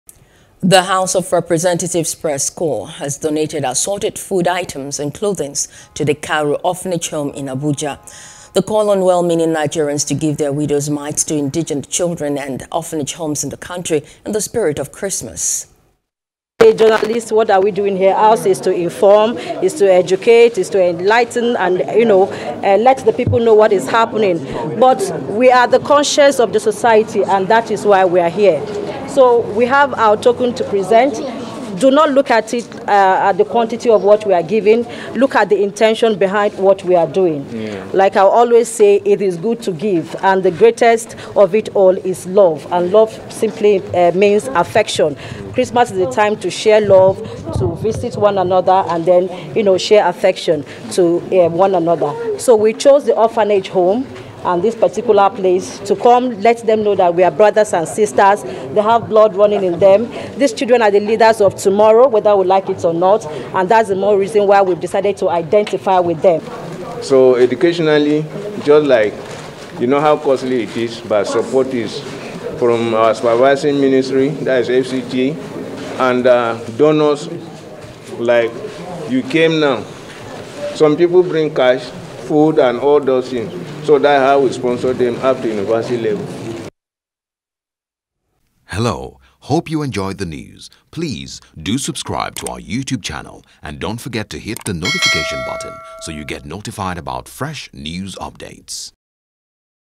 Journalists Distribute Food, Clothing To Orphanage Homes In Abuja
The House of Representatives Press Corps had donated assorted food items and clothing to the Karu Orphanage home in Abuja.